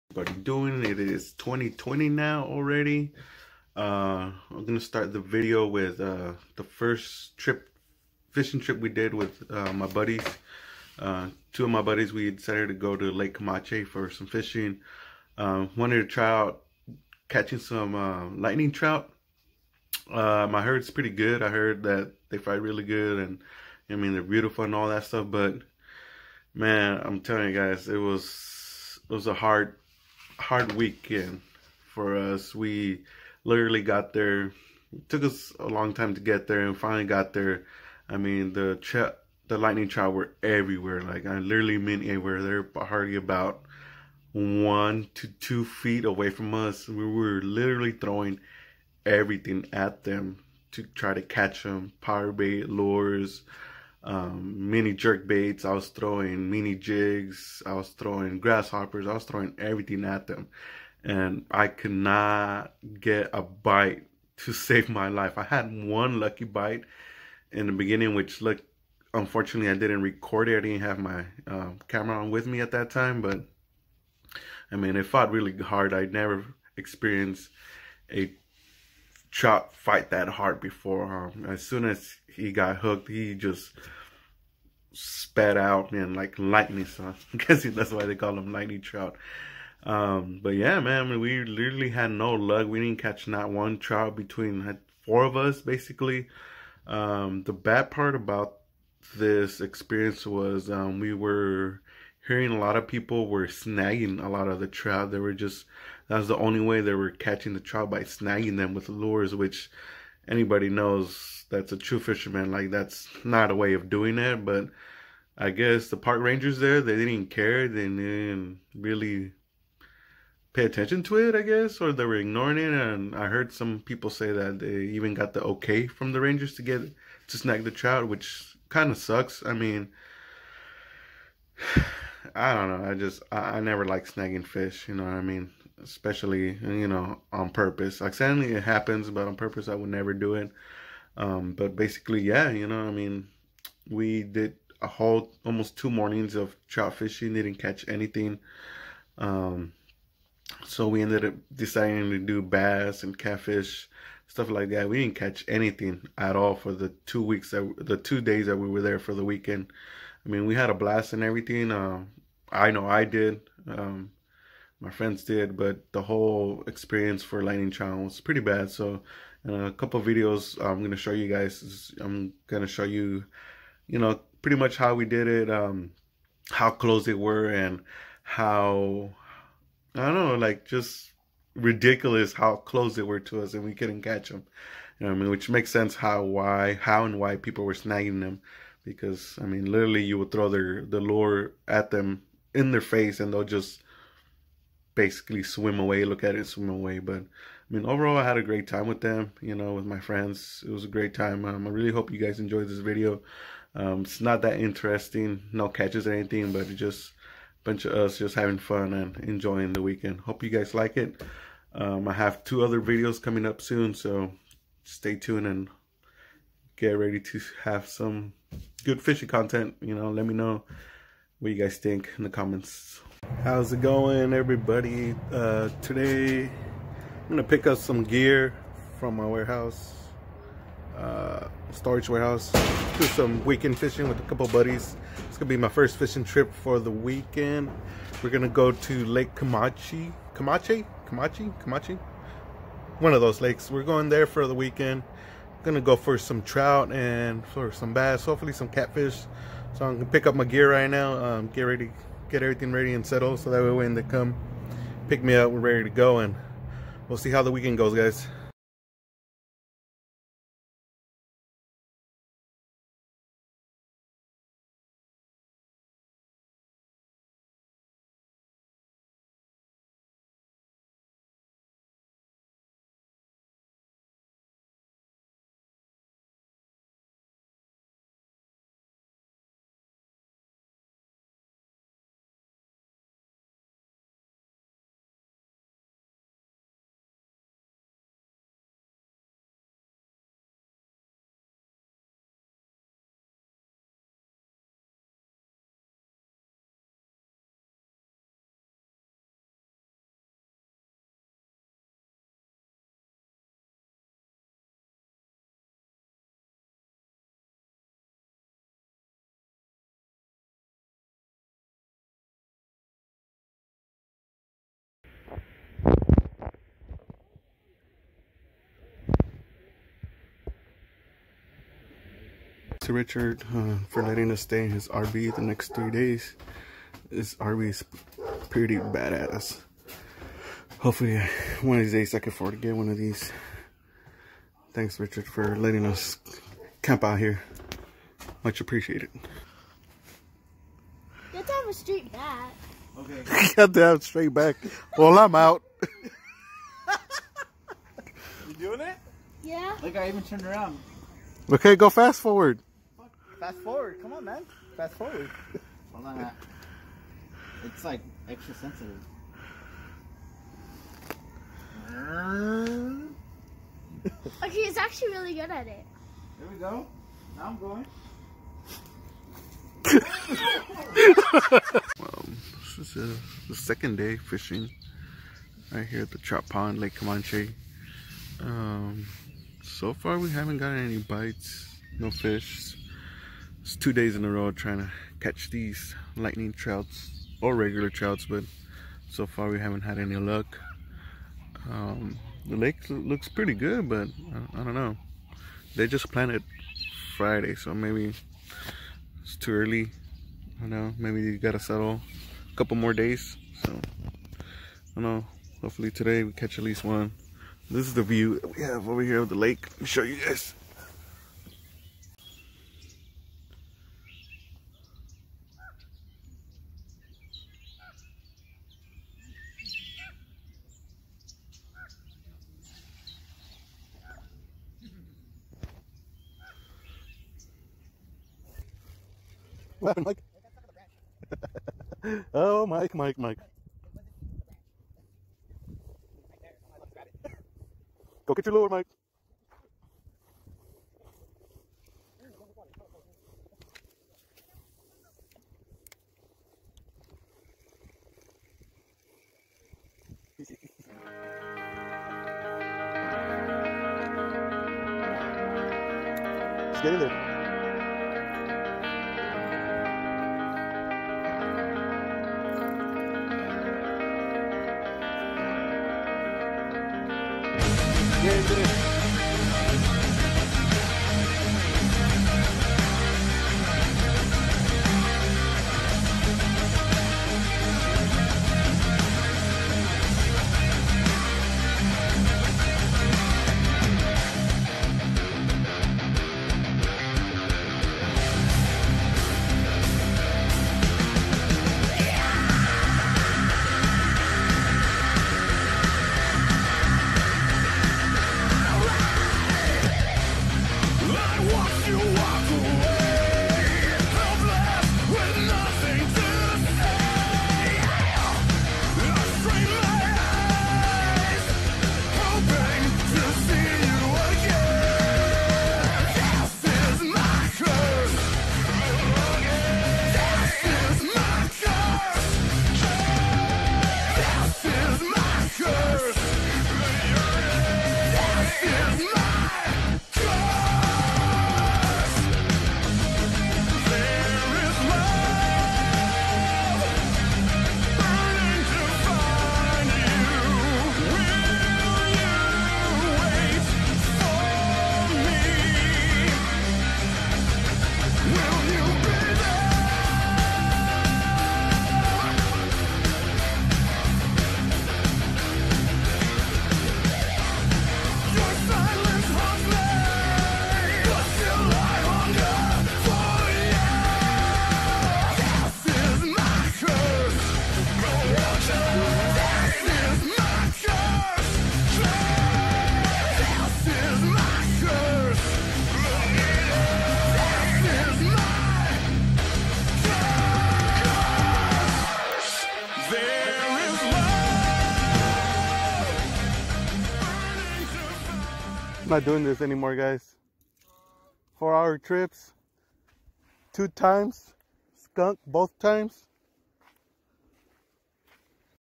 First fishing trip of 2020
Lake camanche
Skunk skunk skunk